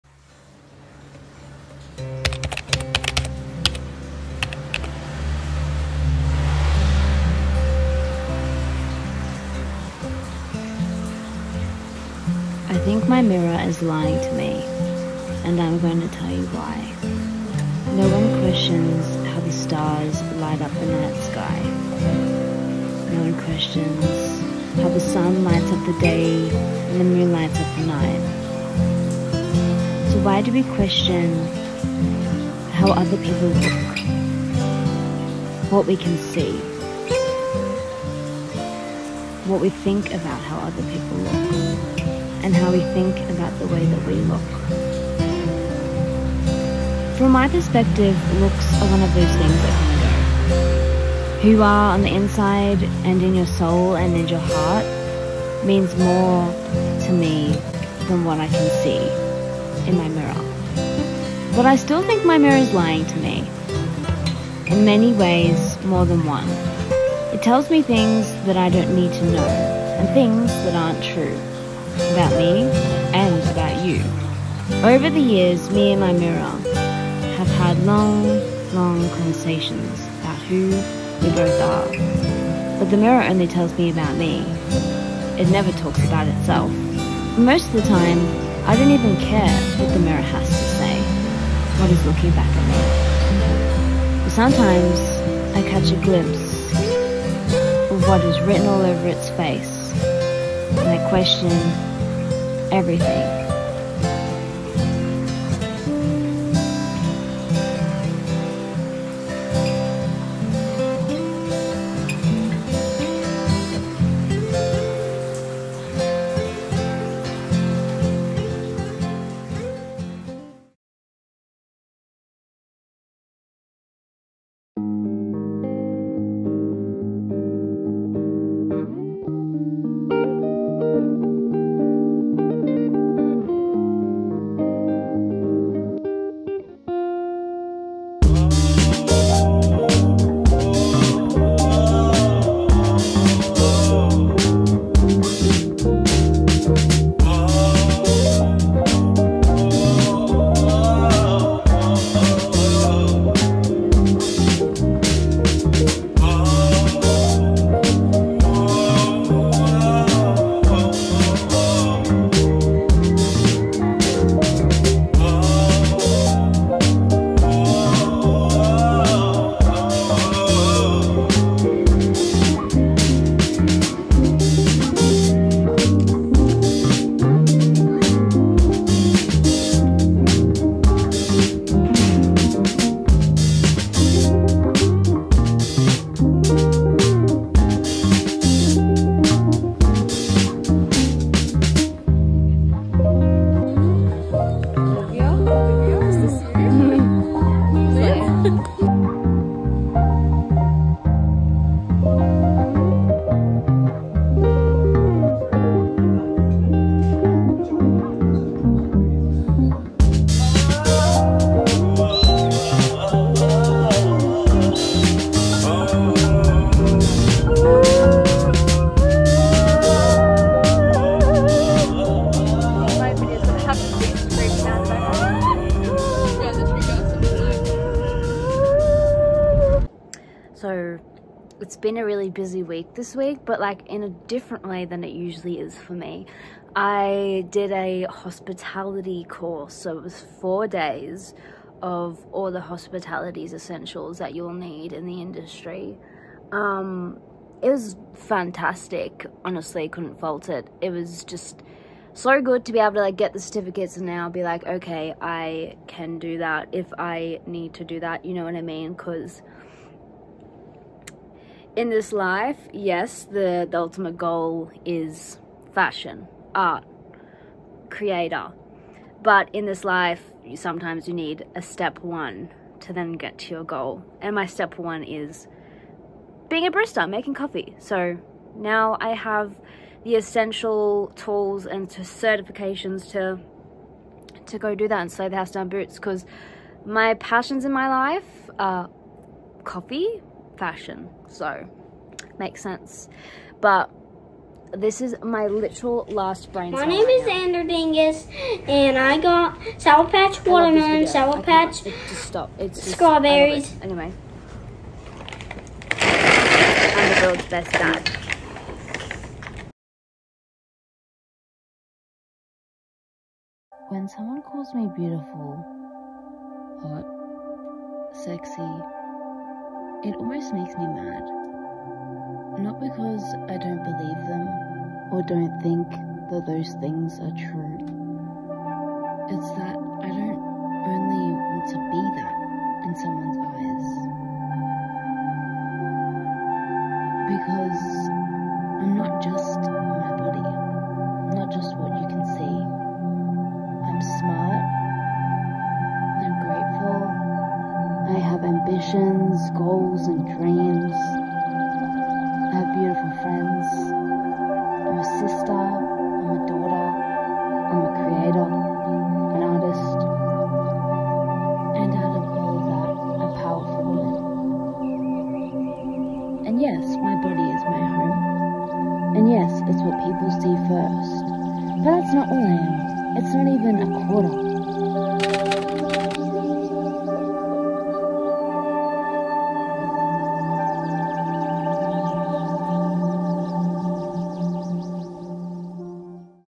i think my mirror is lying to me
☮️ WELCOME ☮️

I think my mirror is lying to me and im going to tell you why.

SOCIALS:
🌼Depop - Madisvintage